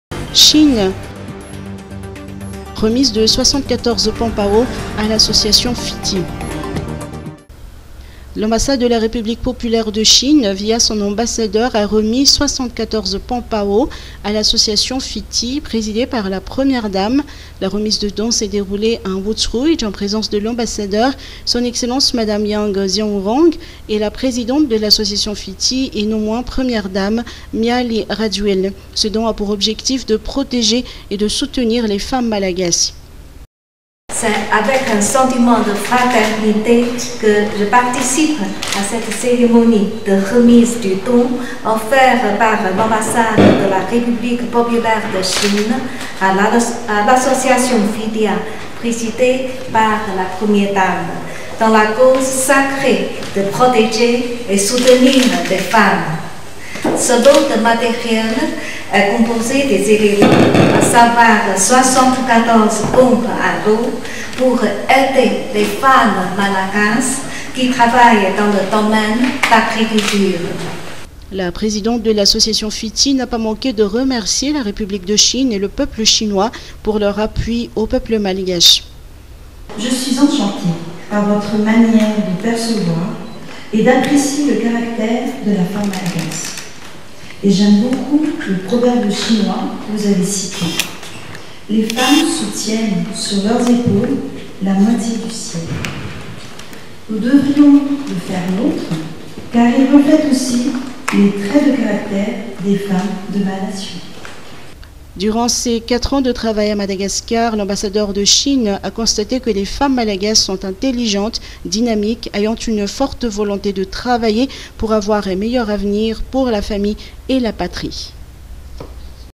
chine
L’Ambassade de la République Populaire de Chine via son Ambassadeur a remis 74 pompes à l’eau à l’Association Fitia présidée par la Première Dame, Mialy Rajoelina.